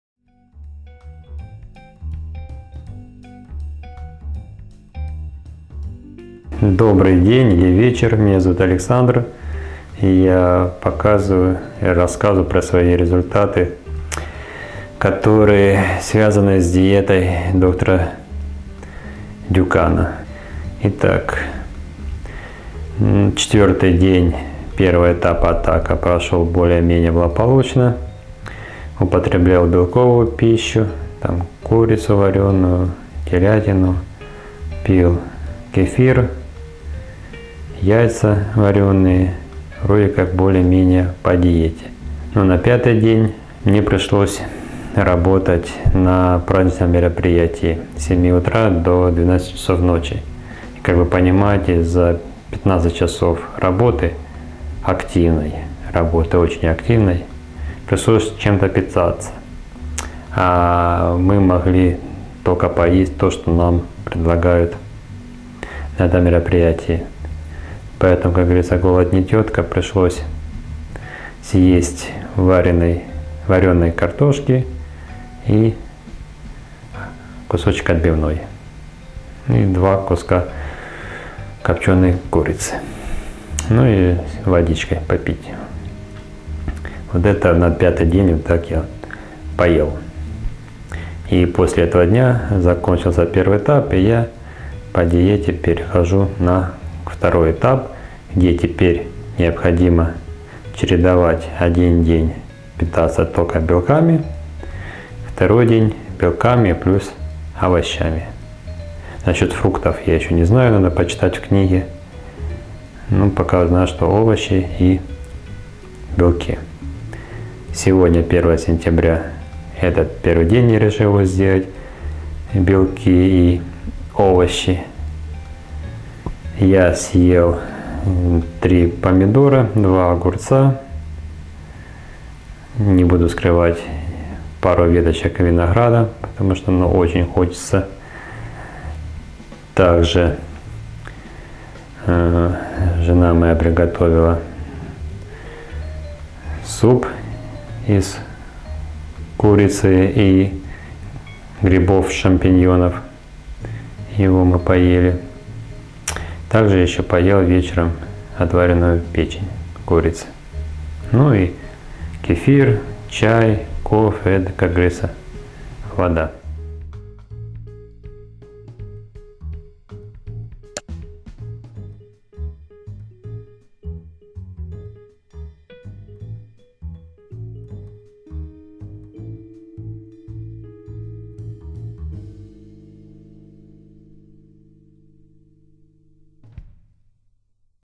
диета доктора пьера дюкана 6 день
Шестой день питаюсь по белковый диете доктора Пьера Дюкана. Захожите на мой видео канал и задавайте любые вопросы.